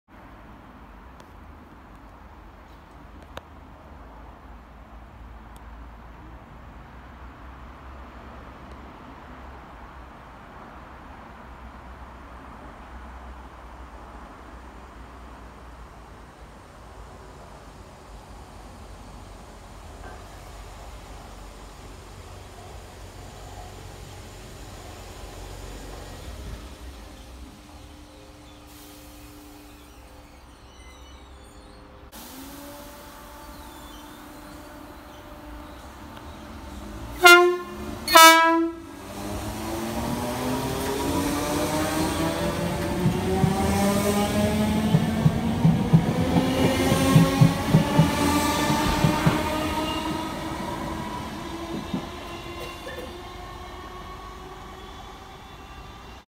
313203 with loud 2 tone horn
313201 in a brighton-Portsmouth and south sea service ariving and departing goring bye sea with a loud 2 tone horn